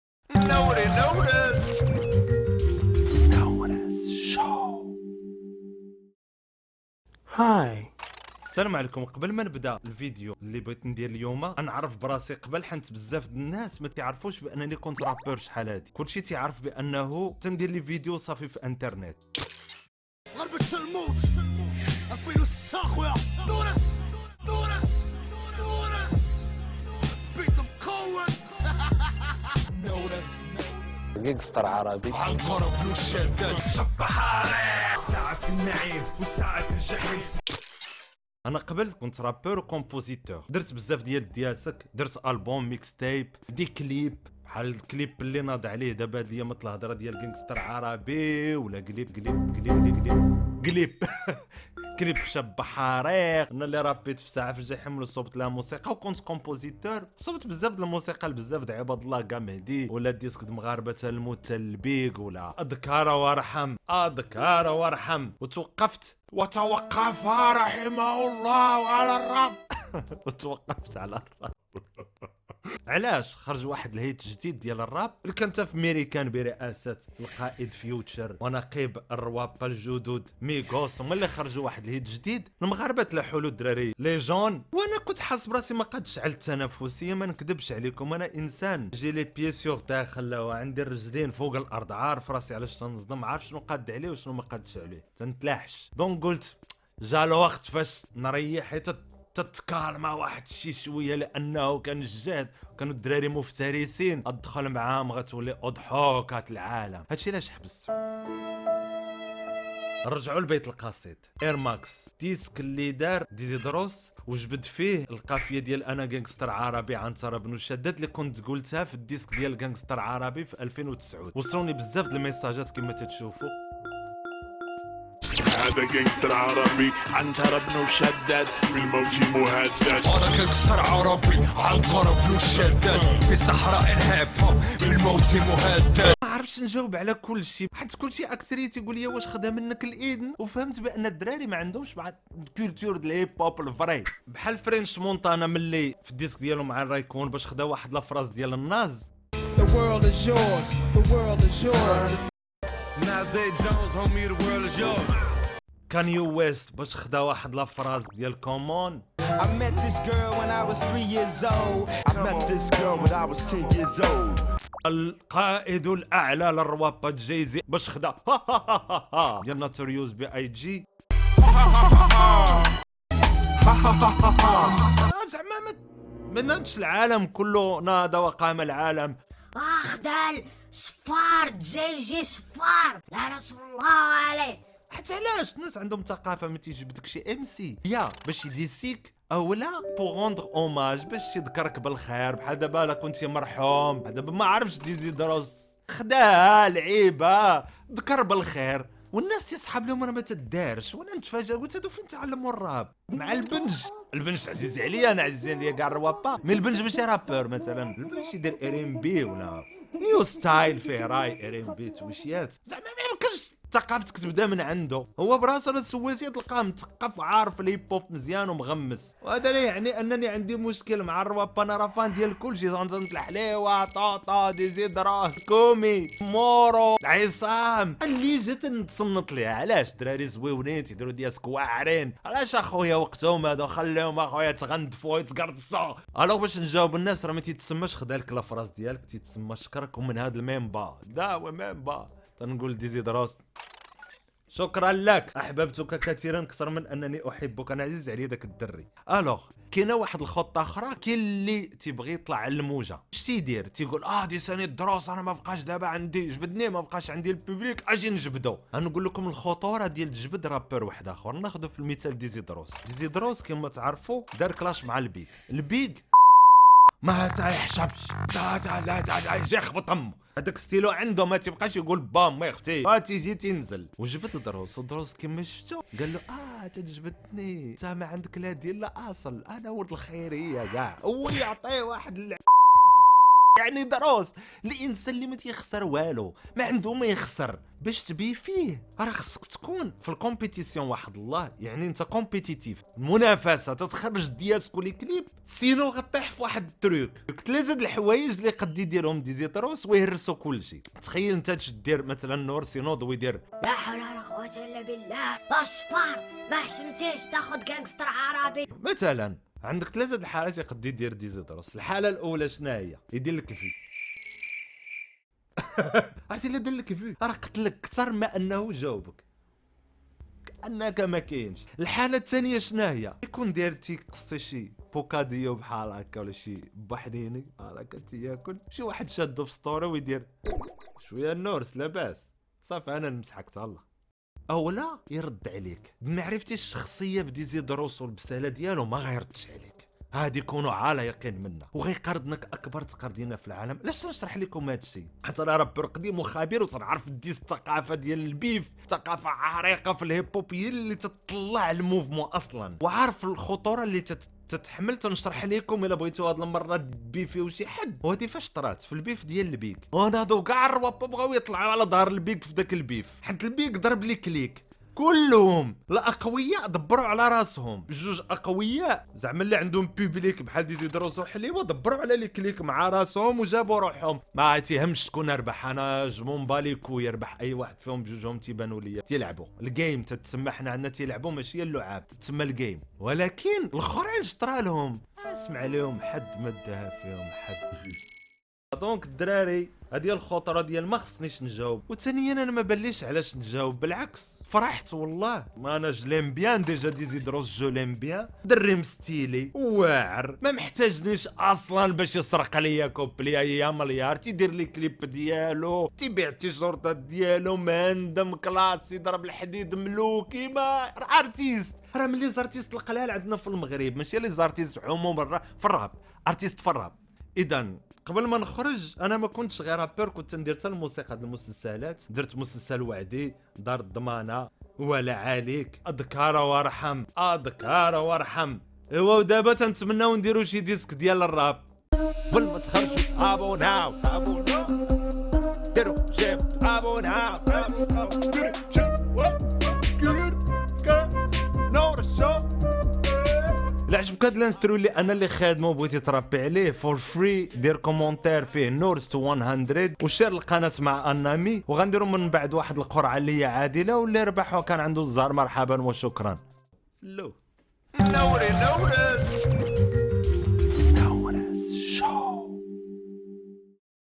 ديزي دروس سرق مني AIRMAX...... وخاصني ننتاقم !!
gangster arabi √

!! و خاصني انتاقم AIRMAX ديزي دروس سرق مني

من أجل التبرعات ديالكم إدا حبيتو المحتوى يبقى ها